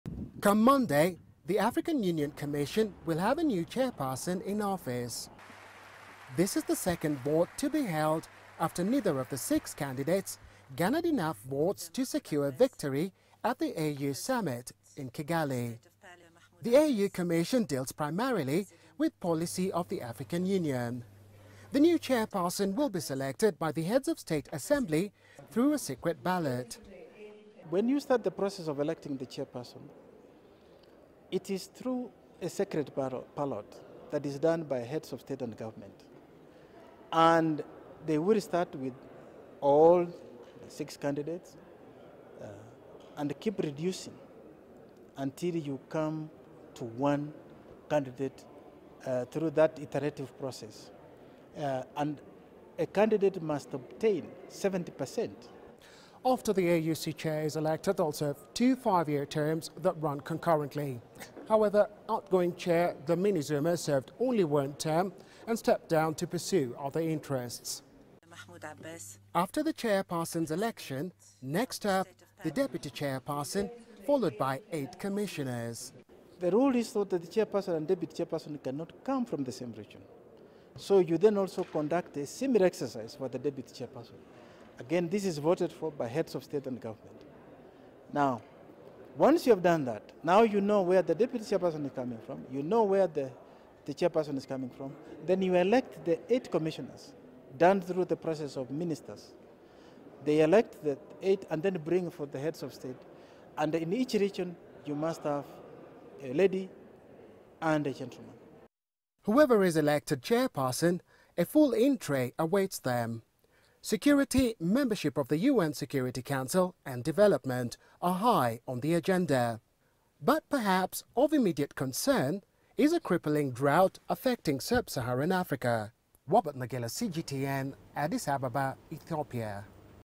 AU chair: Selection process begins for commission's new chair
It's almost D-Day for six candidates, in the running for the position of African Union Commission Chair. Among them the current UN representative for the central African republic Abdoulaye Bathily a Senegalese national, Amina Mohammed, the current foreign affairs minister in Kenya and Botswana's Pelonomi Venson-Moitoi, who fell short of the required votes, in the first elections in July last year. CGTN's Robert Nagila now takes through the process of selecting a new AUC chair.